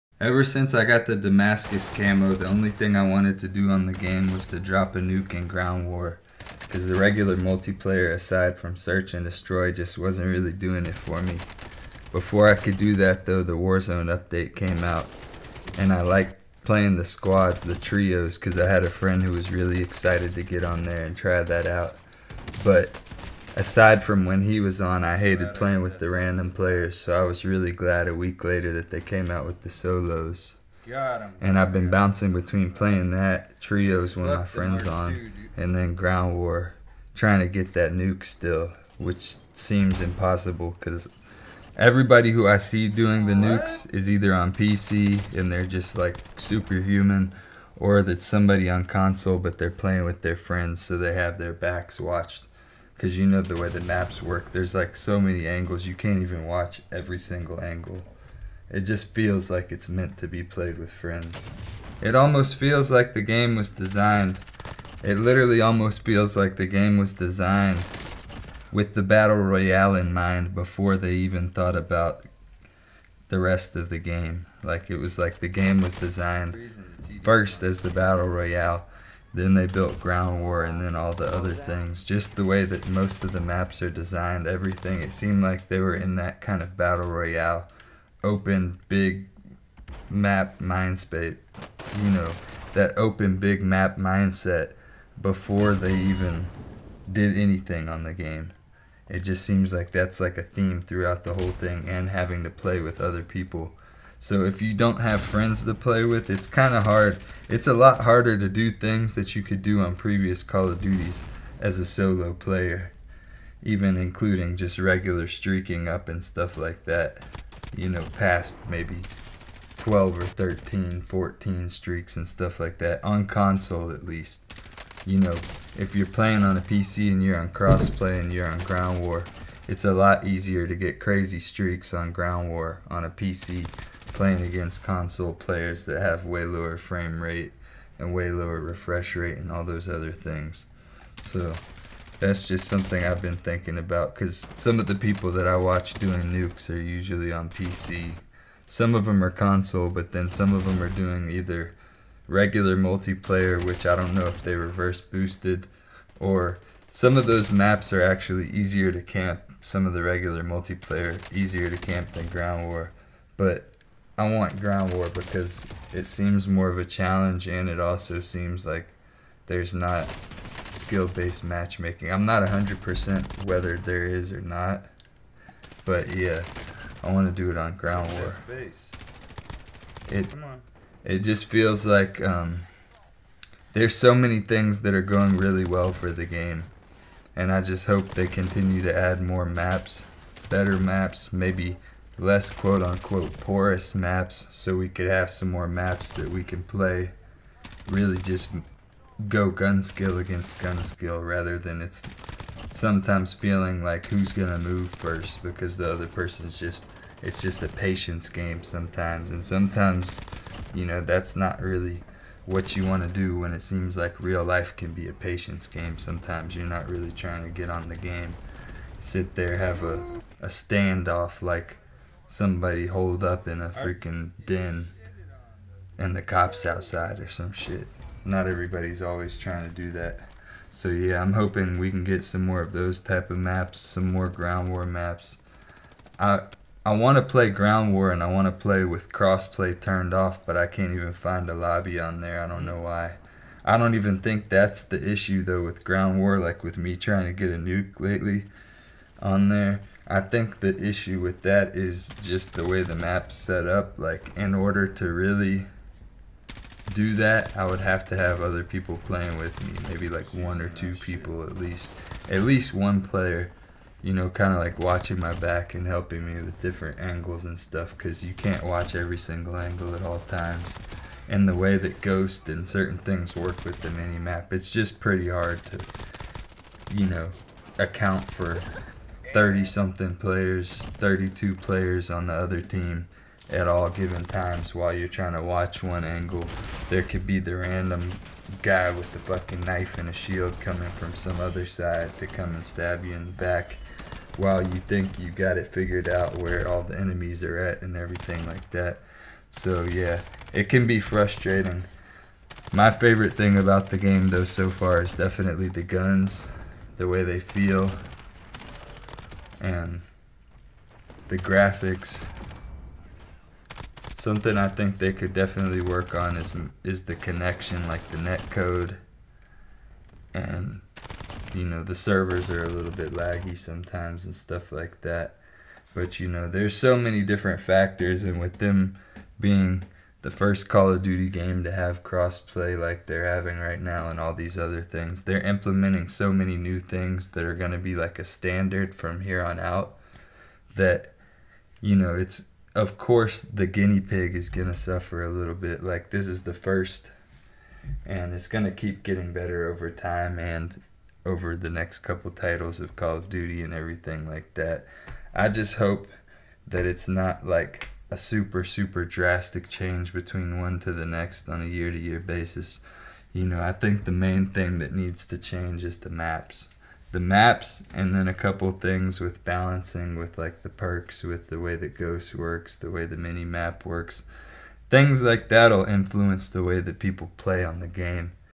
Why People Camp
about halfway through the life cycle of modern warfare, I decided to just speak on my overall thoughts about the game and a little about what I think is to come, what I think could change, etc.  I'm looking forward to seeing what they bring in season 3 as well, let me know your thoughts in the comments section (i know if you really have something to say you don't need me to tell you to do so lol) Everybody have a great day!